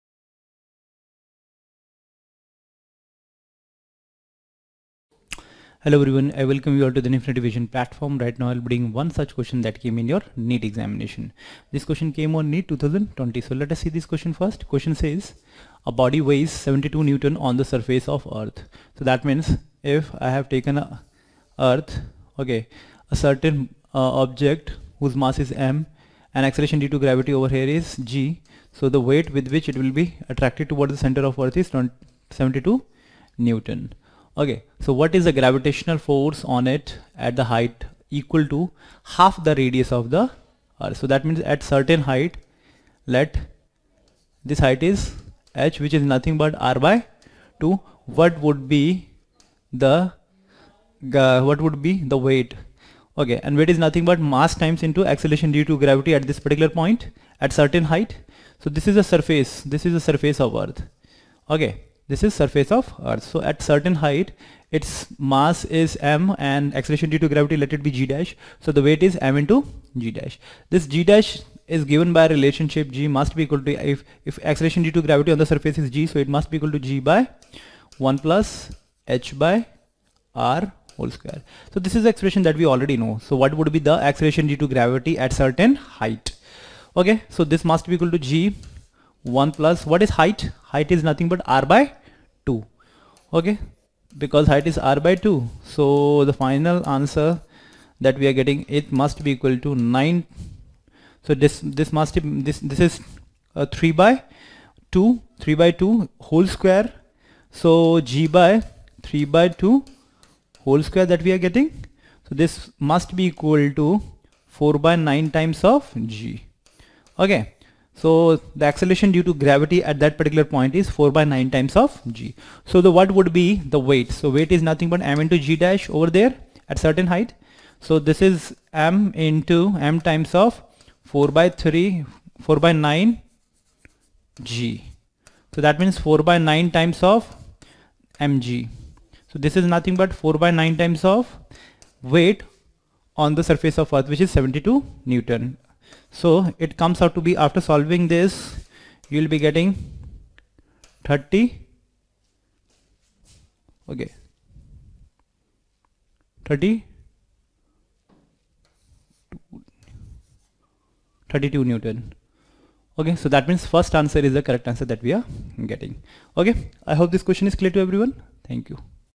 A body weighs 72 N on the surface of the earth. What is the gravitational: Gravitational force
A body weighs 72 N on the surface of the earth. What is the gravitational force on it, at a height equal to half the radius of the earth?